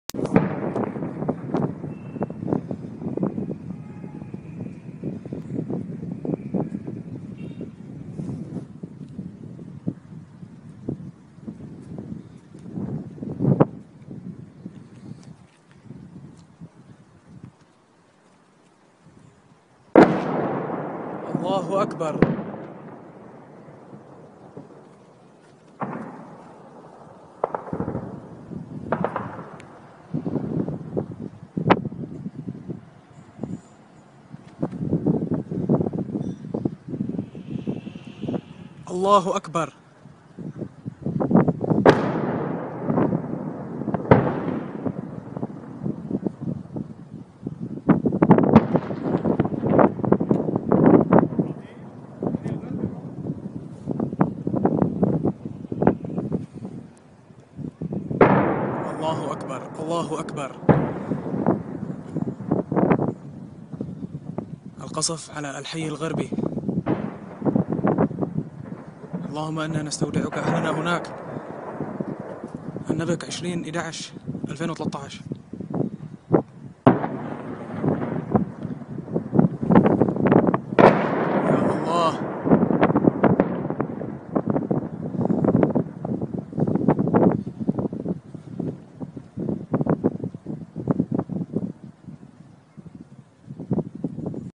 النبك ||20-11-2013|| القصف المدفعي العنيف على المدينة
نقلا عن تجمع احرار النبك-تنسيقية النبك